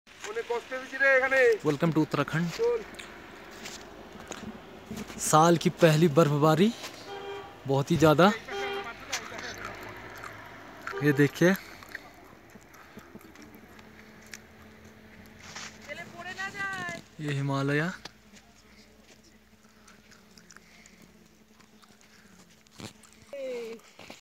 Tourist in munshiyari Uk
me and my brother hemu bhaiya we went to munshiyari this season and really we enjoyed a lot , we were so excited, that day weather was awesome , many bengali family,s were over there . everywhere was covered by snow .